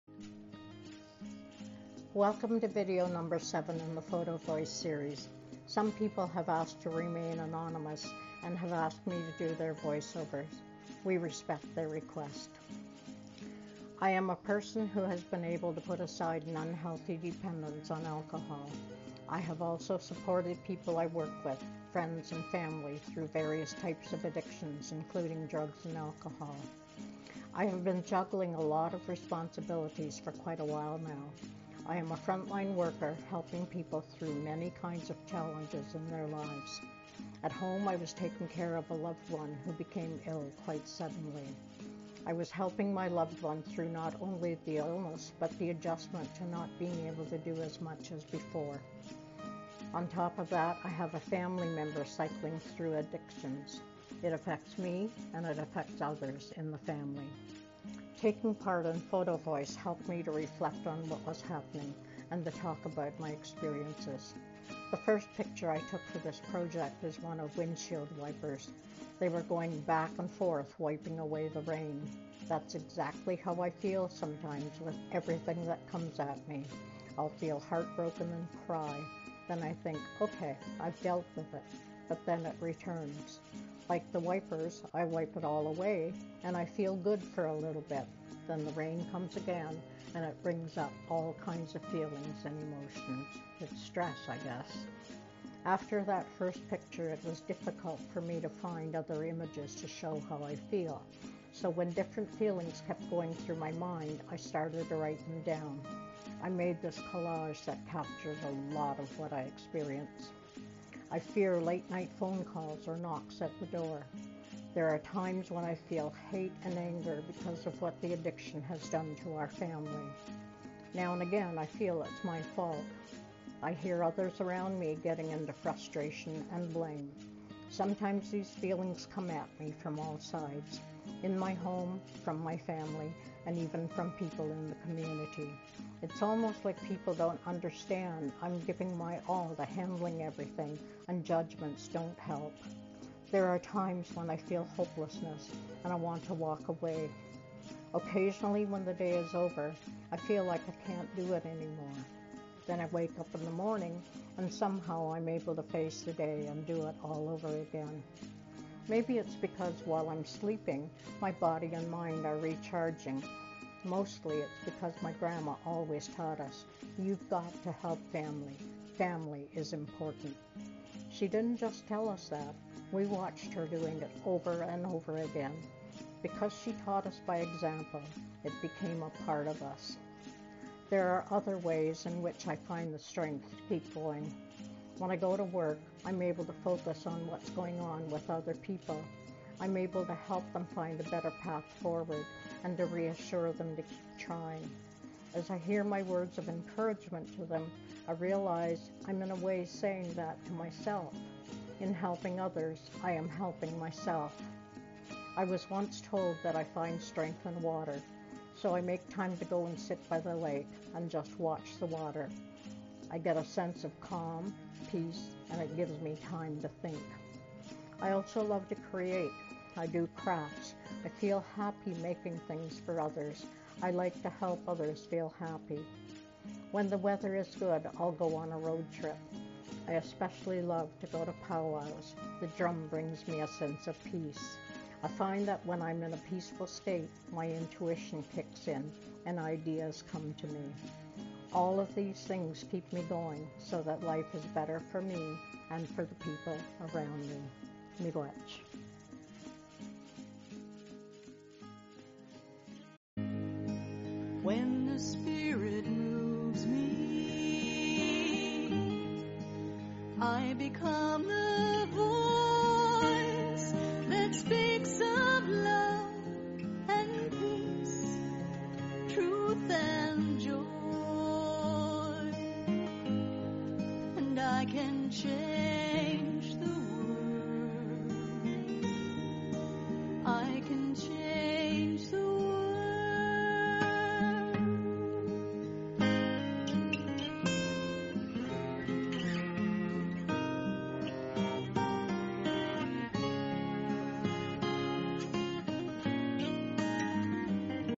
The First Nations Wellness Initiative: Mitigating the Impact of COVID-19 - Video 7
The seventh video in this photovoice series.

A collaboration between the Chippewas of Saugeen First Nation #29 (SFN) and the Centre for Addictions and Mental Health (CAMH) to develop a Community Wellness Strategy for SFN and surrounding areas.

We are all affected in some way by issues with mental health, substance use or violence, and these are just a few of the personal stories that some of our brave community members have agreed to share.

We will be sharing a video each week until the end of the series.